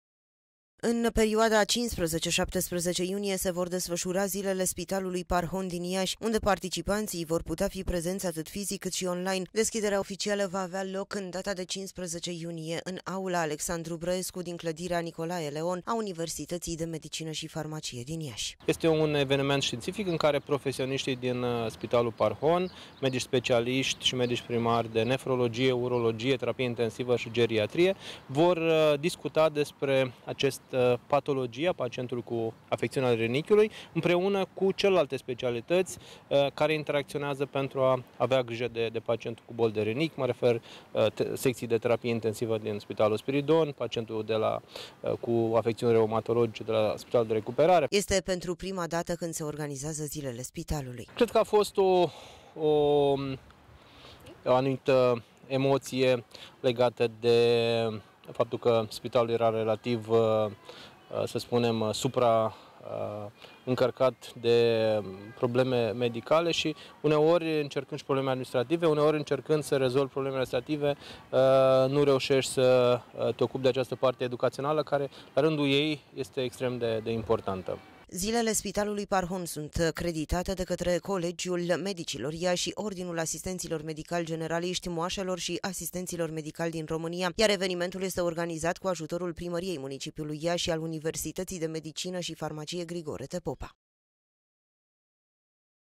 Premiera la Spitalul Parhon Iasi
Premiera la Spitalul Parhon din Iasi. In perioada 15-17 iulie se vor organiza zilele spitalului, in format hibrid,  atat cu prezenta fizica, cat si in mediul online. Evenimentul va fi sprijinit de Universitatea de Medicina si Farmacie si Primaria municipiului Iasi. Gazda evenimentului va fi cladirea Nicolae Leon din cadrul UMF unde vor fi prezenti peste 200 de participanti, printre care si
speakeri internationali si nationali, experti locali, si formatori de opinie in mai multe domenii medicale.